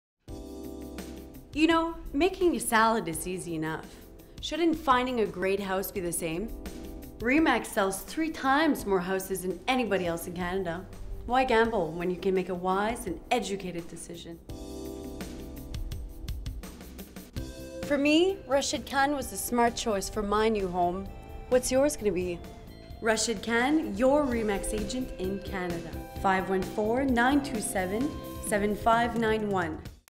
Remax-Kitchen FINAL Hi-Res.mov
The New REMAX-KITCHEN Commercial from Soner Rock Films and Rayman Films:

Ashley Jane as The Buyer;
Rashid Khan as The Agent;
Bablu MD Lutfor Rahman - Director;
Sanjidha Bably - Assistant Director;
Joseph Antoine Clavet - Cinematographer;
Ian Donald - Key Grip;
José Claudio Cunha e Silva - Sound and Music Recordist;
Jonathan Charlebois - Production Assistant;
Carolyn DeKoninck - After Effects;
John Lipsz - Color Editor;
Daniel Roy - Writer and Editor;

PRODUCED BY Daniel Roy and Bablu MD Luthfor Rahman